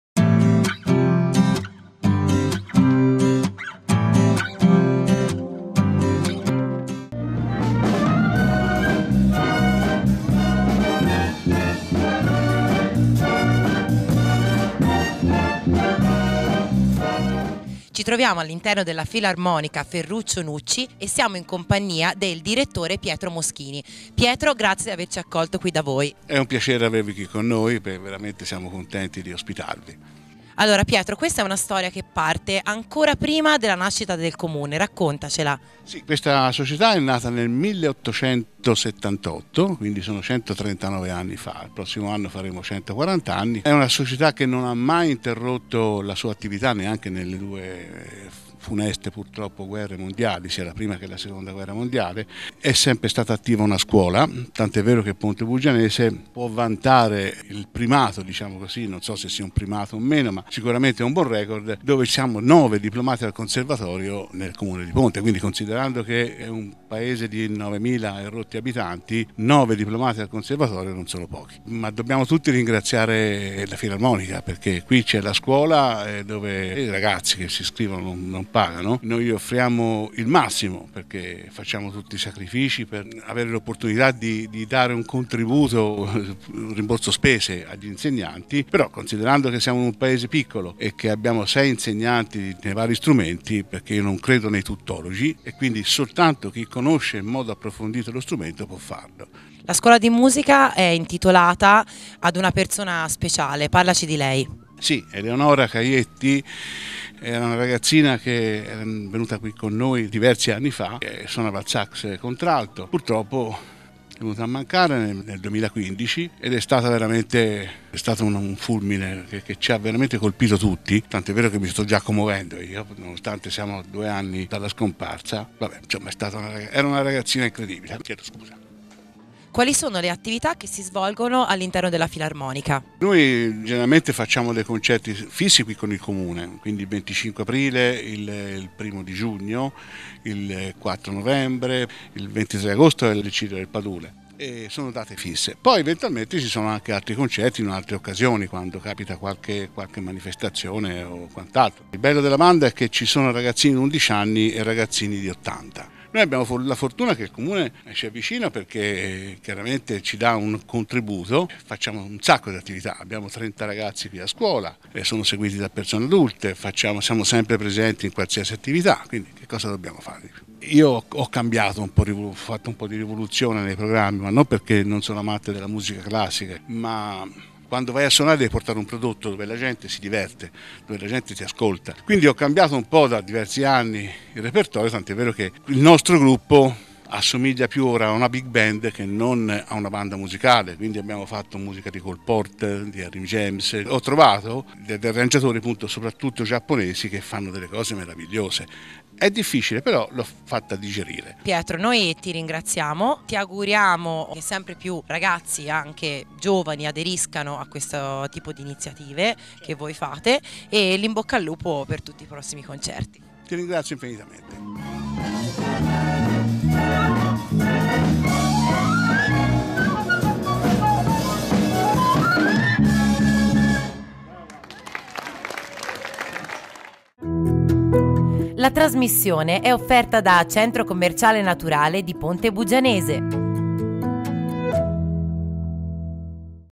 La Filarmonica F. Nucci di Ponte Buggianese
Intervista al Direttore Pietro Moschini.
E' ormai un'istituzione in paese per il suo impegno sociale e di rappresentanza dell'intero territorio.
La Società Filarmonica “Ferruccio Nucci” del presidente Luca Banditori riparte quest'anno con nuovi stimoli e tante iniziative.
Grazie al contributo della Fondazione Cassa di Risparmio di Pistoia e Lucchesia, la sede di via della Libertà si presenta rinnovata e dotata di tutti i confort necessari per l'attività dell'associazione.
É stato infatti ripristinato l'impianto di climatizzazione all'interno dei locali e sono stati acquistati il “labaro” di rappresentanza, essenziale per evidenziare la partecipazione della Filarmonica alle varie manifestazioni pubbliche, e le nuove camicie della divisa, necessarie per vestire le nuove leve dell’organico formate nei corsi musicali della Scuola della Banda.
Proprio in riferimento ai corsi di quest'anno, sono aperte le iscrizioni per l'insegnamento di strumenti a orientamento bandistico, dal pianoforte fino alla batteria.
I docenti, tutti diplomati, durante le lezioni potranno avvalersi anche della collaborazione di Vincenzo Genovese, apprezzato insegnante di pianoforte e di musica d’insieme, nonché attuale Direttore Artistico del Gambrinus Music Cafè di Montecatini.

La Filarmonica “Nucci”, nel ringraziare i cittadini e le istituzioni che da sempre la sostengono e la accompagnano nella propria attività, invita chi fosse interessato a partecipare ai corsi di musica
Da ricordare che il maestro Riccardo Muti, grande direttore d’orchestra, ha sempre ricordato l’importanza che hanno in Italia le bande per l’educazione musicale.